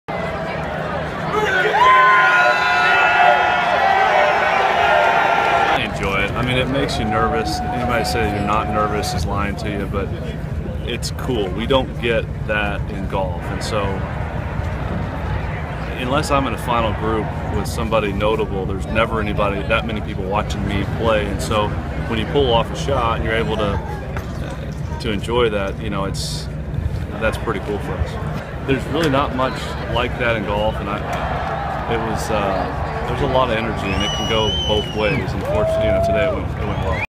The Greatest Show On Grass
Booze and boos: The 16th hole at the Phoenix Open has become golf's version of a mosh pit. (Feb. 11, 2016)